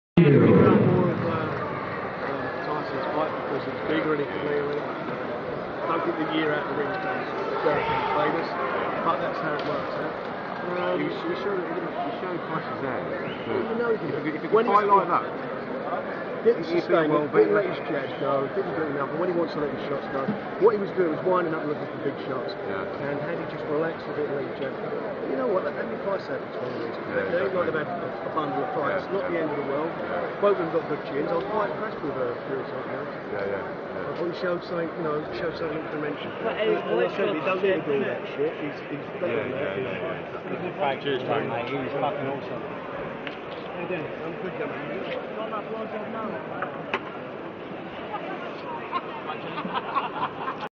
Frank Warren talks Tyson Fury v Derek Chisora
Frank Warren talks about the loss of his fighter Chisora at The "Big Brawl" Wembley London, Fury defeated Chisora and won by UD for the British Heavyweight & Commonwealth Ttile reported by Samaarah Choubak